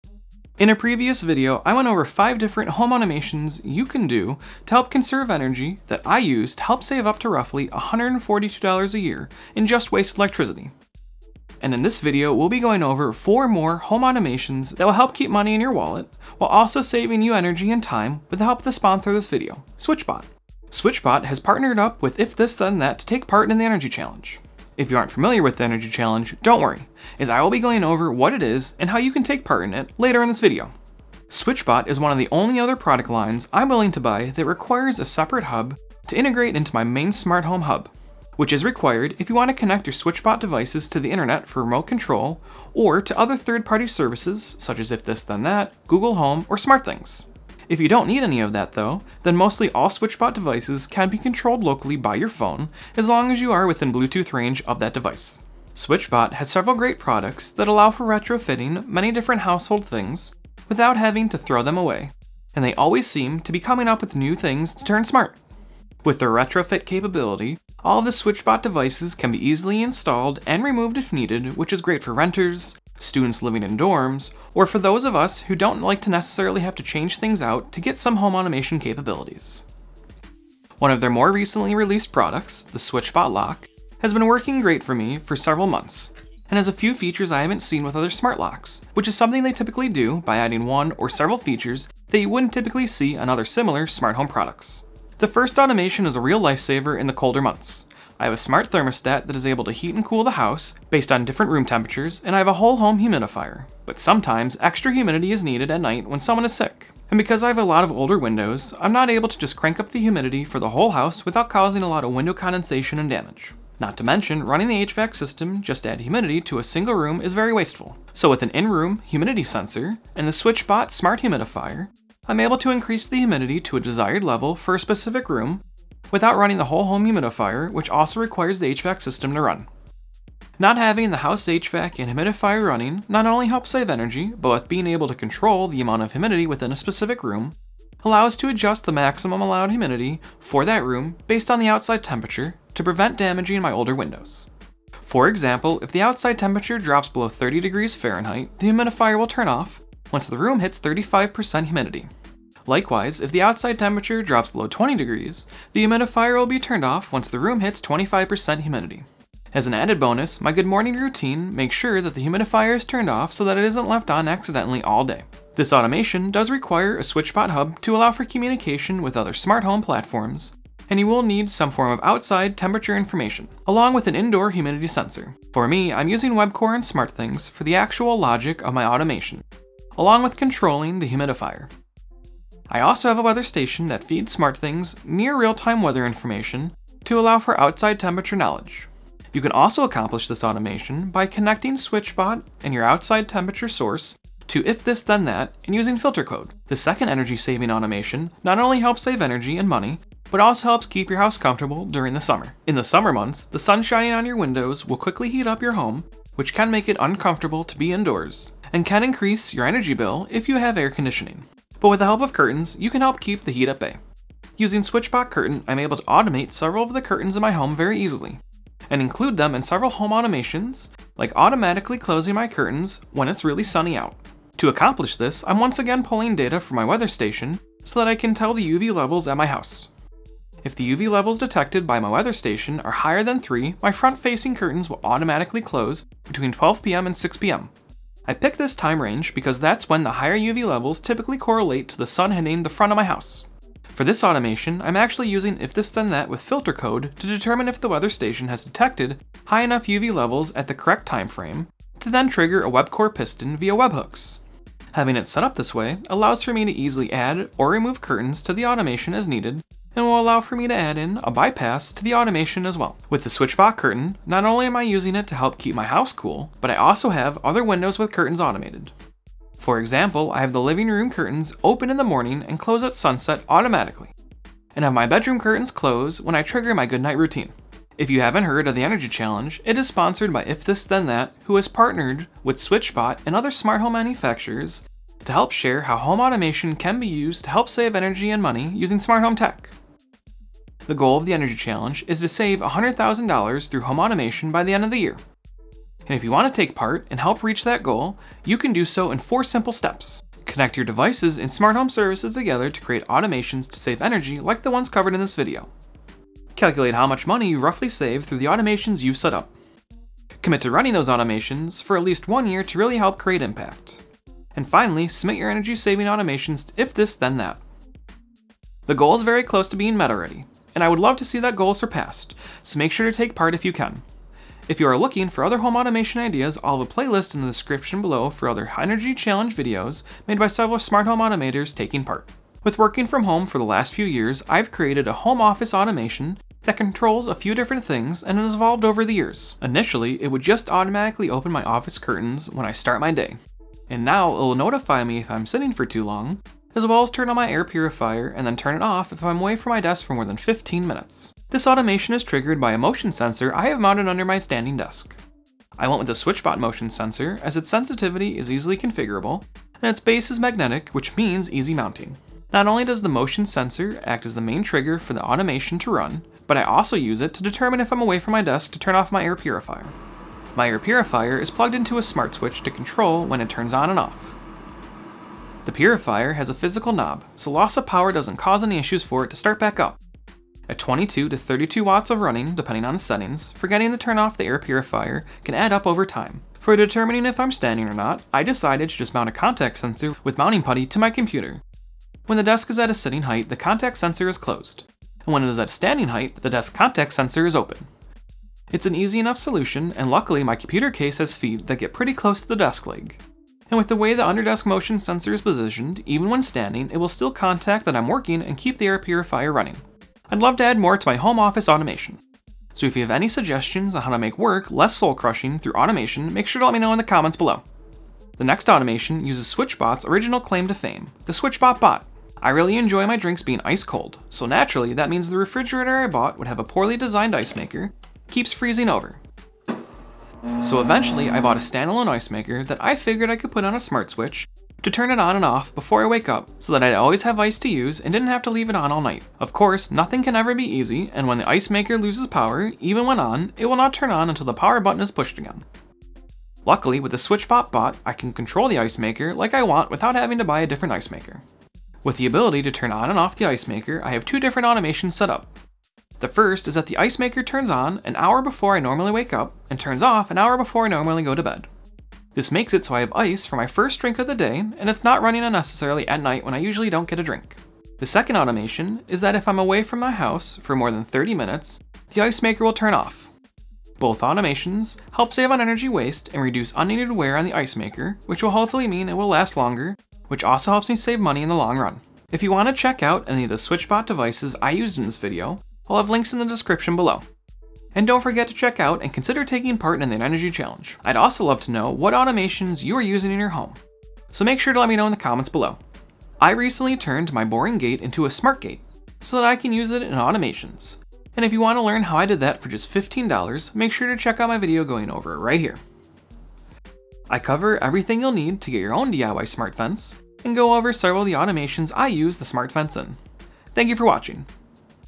Save Money (And Energy) with Home Automation ⚡
In this video we will be going over several different smart home automations YOU can setup to save energy, money, and time. I'll also be going over how you can take part in the energychallenge if you aren't already.

🛒SwitchBot Hub
🛒SwitchBot Meter
🛒SwitchBot Bot
🛒SwitchBot Motion Sensor
🛒SwitchBot Curtain
🛒SwitchBot Smart Humidifier

Chapters:
0:00 - How To Save Money (And Energy) with Home Automation
1:34 - Smart Home Room Comfort Control
3:15 - How To Save Money With Your Curtains & Home Automation
5:31 - Using Your Smart Home To Making Working From Home More Enjoyable
6:57 - How To Make Old Appliances Smart